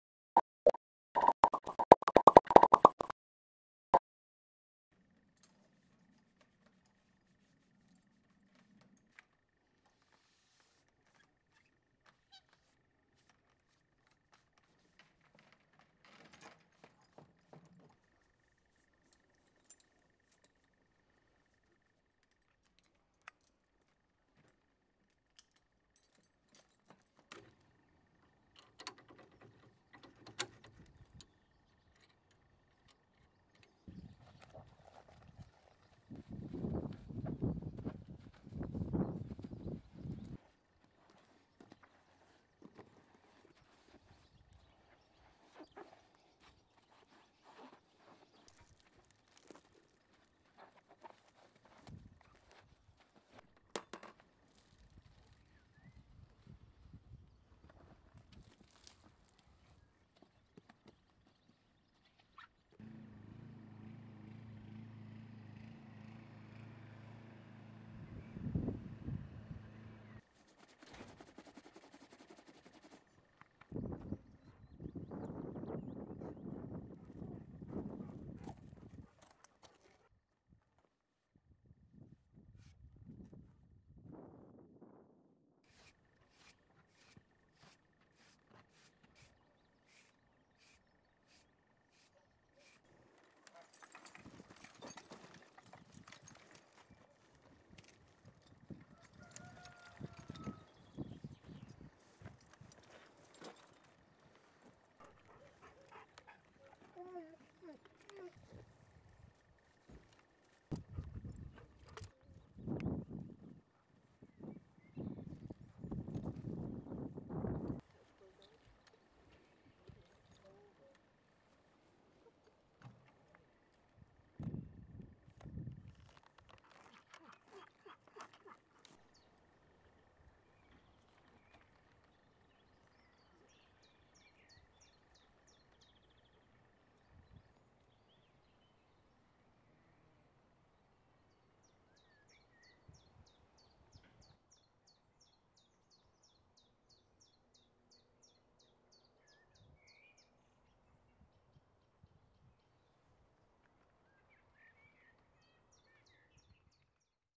Making Our Homemade Horsedrawn Hearse. Part Three: Finished!
This is part three of the build. I did film quite a lot of the painting, but when it came to putting in the coachlines/pinstripes I found it hard enough without a camera looking over my shoulder too.

So here it is, ready to go. We hope you don't need our services too soon, but if you're in our County Cork, and you're interested then do get in touch. We didn't make it just to leave it under a dust cover in the barn.